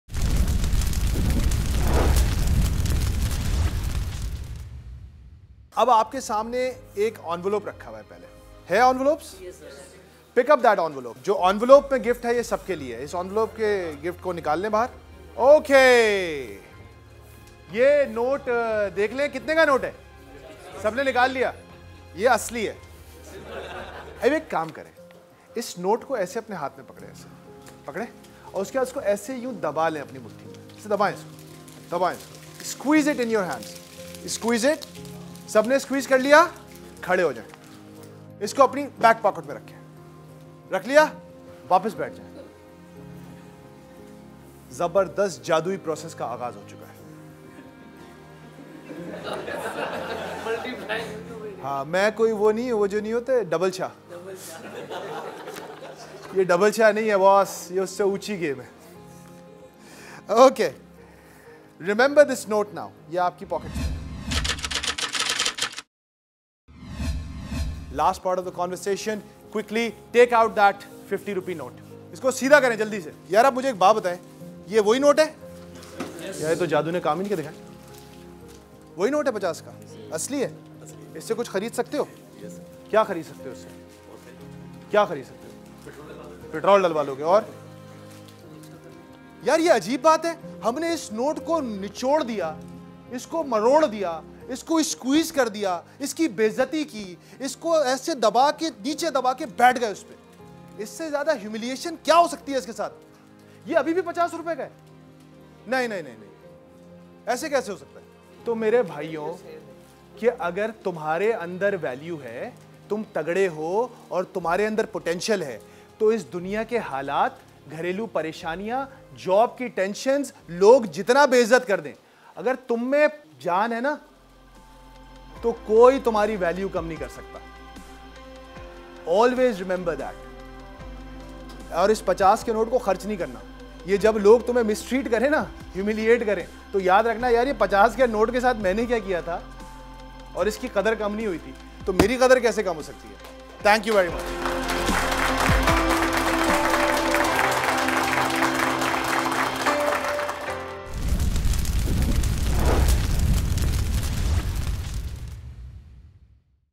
See how a 50 Rupee note can change your life? Help you fight against all ODDS. Firestarter explains
حالات خراب ہیں، دل پریشان ہے، ساری مصیبتیں ایک ساتھ ہی کیوں آرہی ہیں؟ ہر راستہ بند ہورہا ہے؟

اگر آپ کو کبھی بھی ایسا محسوس ہوا ہو تو دیکھیں کہ کیسے ایک پچاس روپے کا نوٹ آپ کو زندگی کا سب سے قیمتی سبق سکھا سکتا ہے